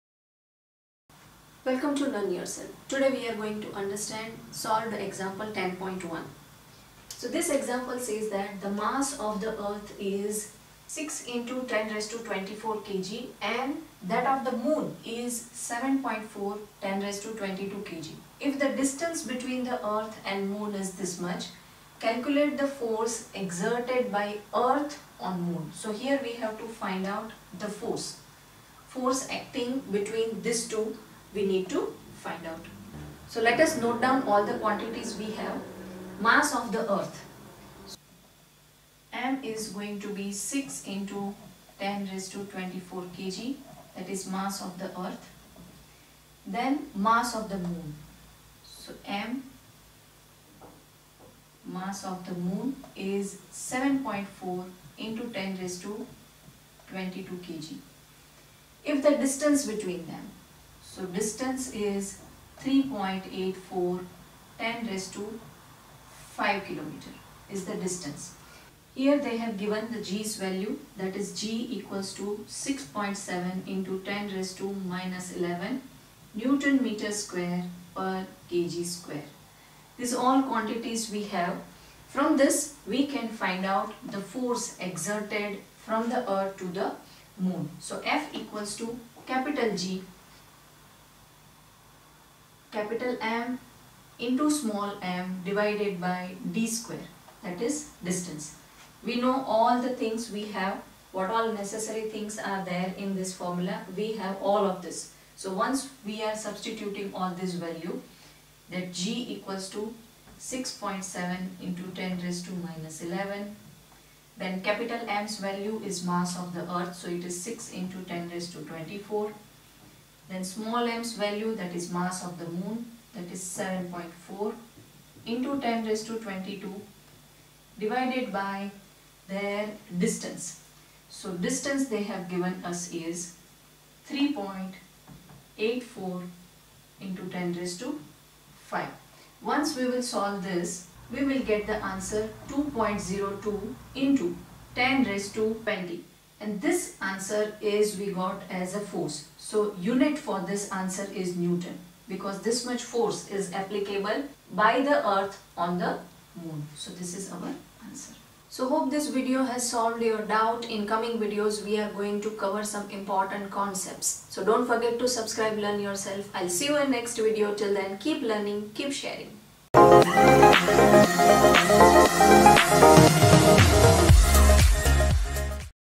CBSE Std-9 Science Ch-10 || Solved Example 10.1 with calculation IN DESCRIPTION || gravitation
The mass of the earth is 6 × 10^24 kg and that of the moon is
7.4 × 10^22 kg. If the distance between the earth and the moon is 3.84×105 km,calculate the force exerted by the earth on the moon. (Take G = 6.7 × 10–11 N m^2 kg^-2)

Calculation Part=

 6.7 x 10^-11 x 6 x 10^24 x 7.4 x 10^22 / 3.84 x 10^8 x 3.84 x 10^8

= { 6.7 x 6 x 7.4 /   3.84 x 3.84  }   x   {10^-11 x 10^24 x 10^22 / 10^8 x 10^8}

= { 297.48 /14.75}  x   {10^-11+24+22-8-8 }

=2.01 x 10^20 N